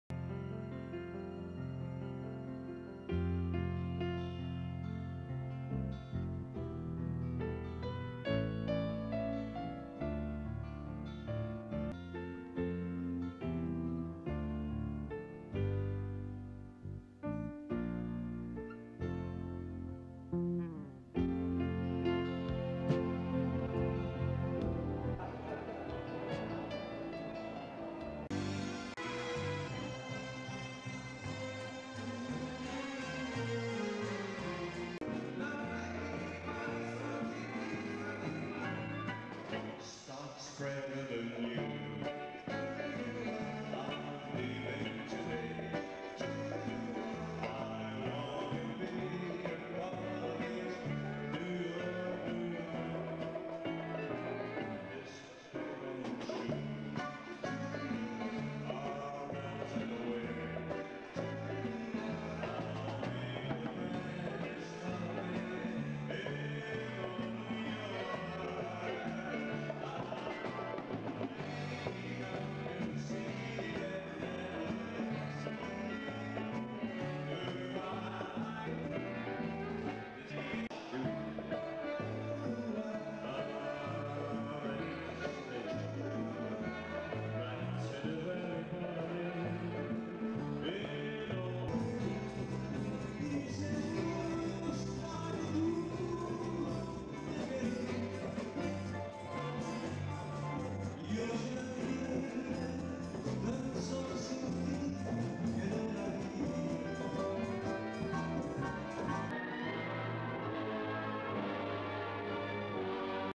Primeiro casamento partes do conjunto Arpége 1985.
Primeiro casamento destaque para conjunto Arpége 23 de fevereiro 1985.